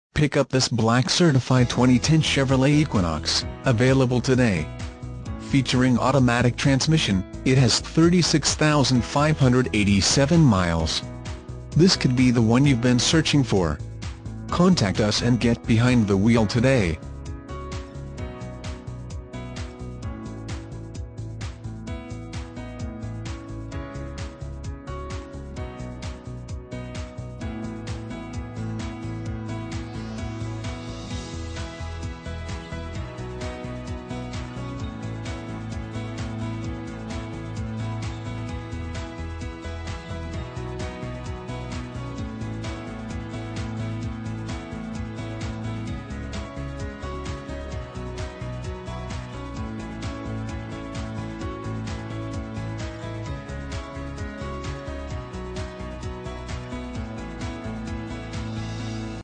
2010 CHEVROLET EQUINOX Minster, OH 20032
2010 CHEVROLET EQUINOX Minster, OH
Stock #20032

Hausfeld Motor Sales, Inc
300 North Main St
Minster, OH 45865

Pick up this Black Certified 2010 Chevrolet Equinox, available today. Featuring automatic transmission, it has 36587 miles. This could be the one you've been searching for.